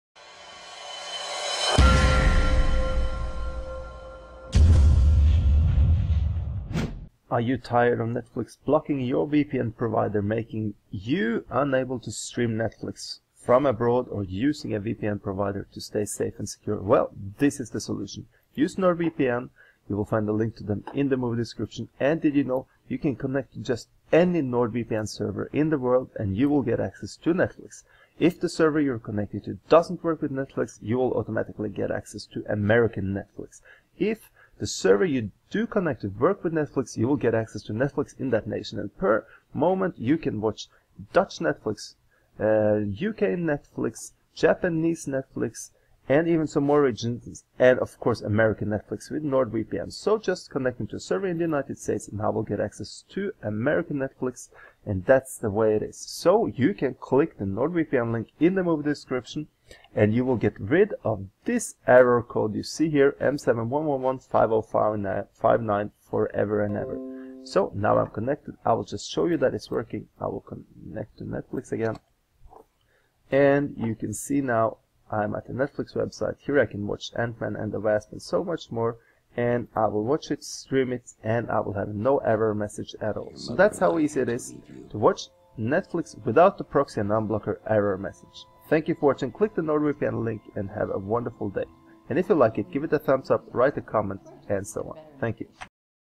Proxy and unblocker error M7111-5059 on Netflix - Bye Bye!
It is time to wave goodbye to the proxy and unblocker error M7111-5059 on Netflix... without waving goodbye to security and encryption. This is how.

It is really easy to get rid of it, but in the jungle of VPN providers, it can feel quite hard to find the one that will actually do the job for you. But, you do not have to look any further, because this is how it can be done! Follow the instructions in the video, or you can also read the following article which will give you information about why NordVPN is the best of all VPN providers if you want to unblock Netflix and get rid of all proxy and unblocker messages you might get.